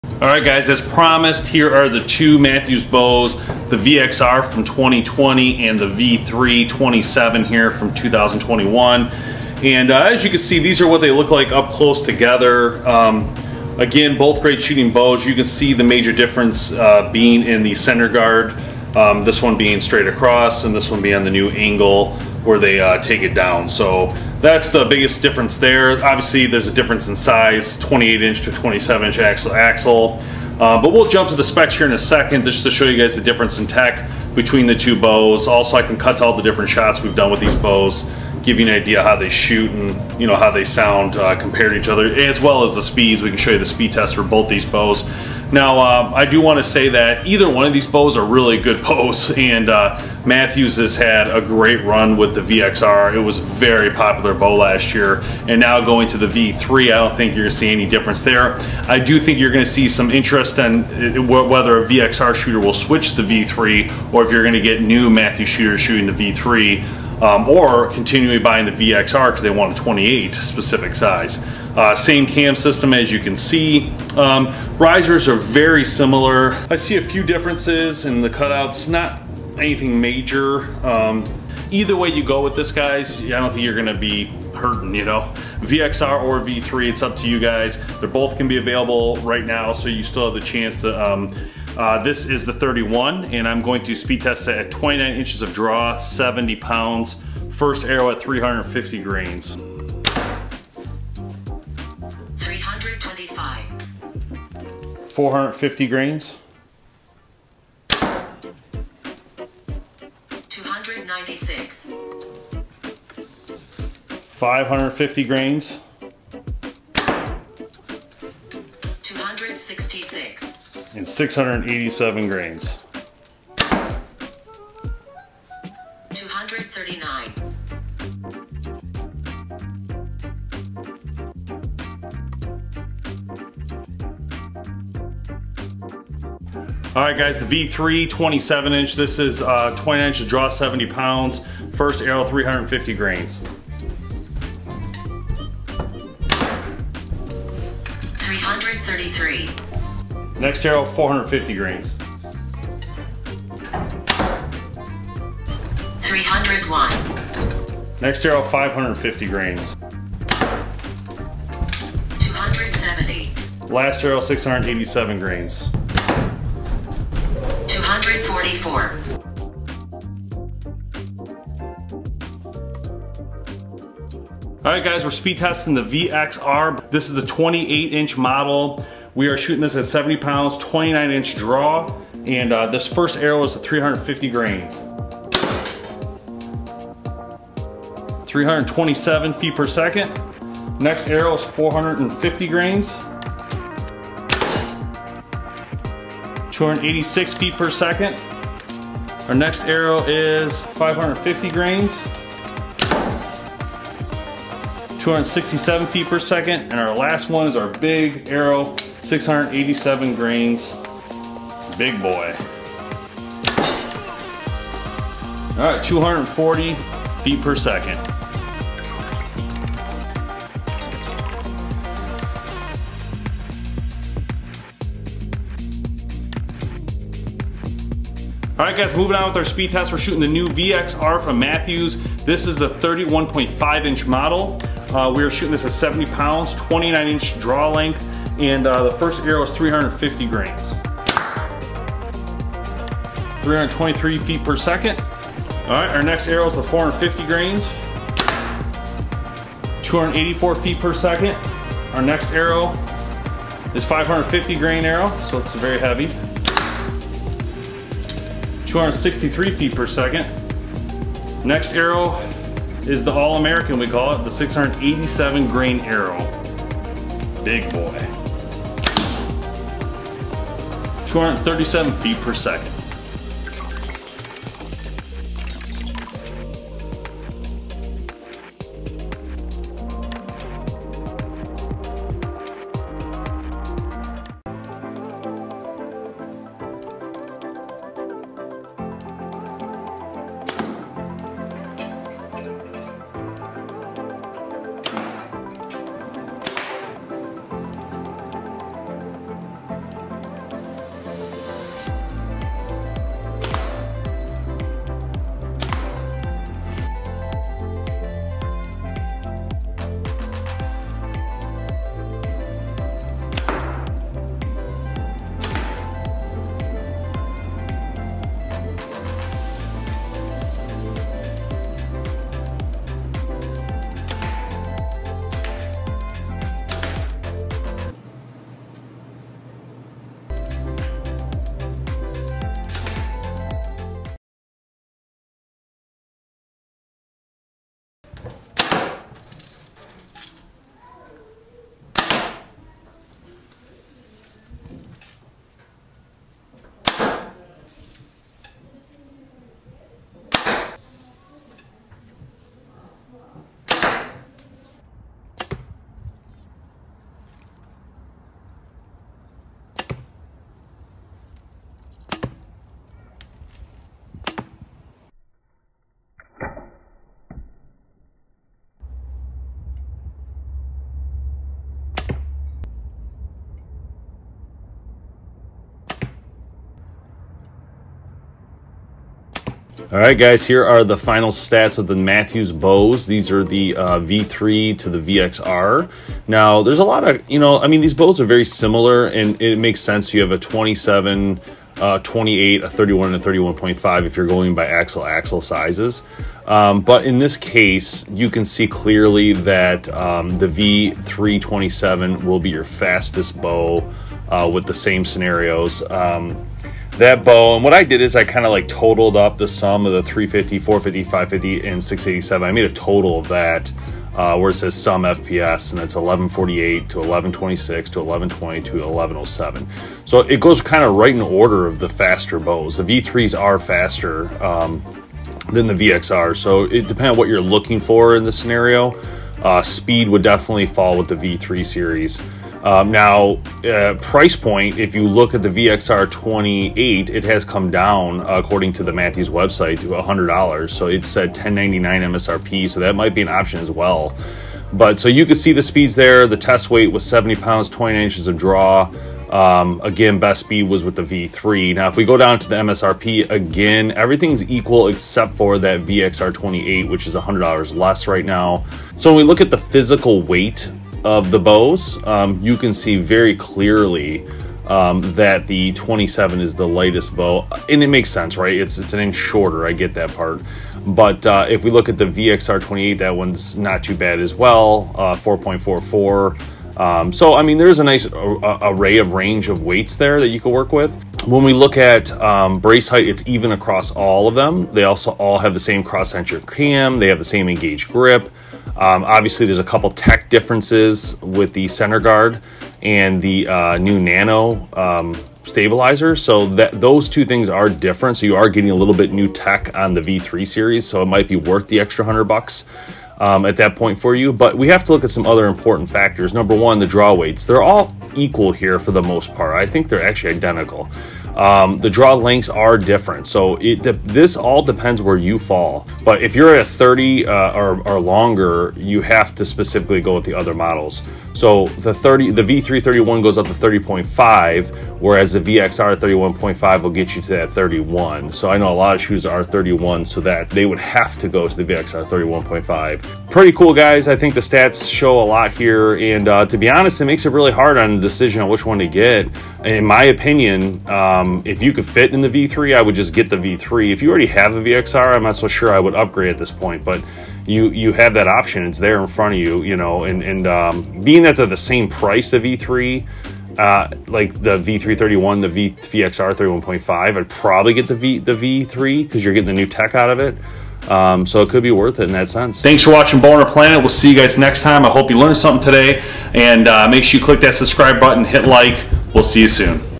NEW! MATHEWS V3 vs VXR
On this episode of BowHunterPlanet we look at NEW 2021 MATHEWS V3 vs 2020 VXR.  As promised we created a comparison video along with speed testing to help the buyer make a decision on which model fits them best.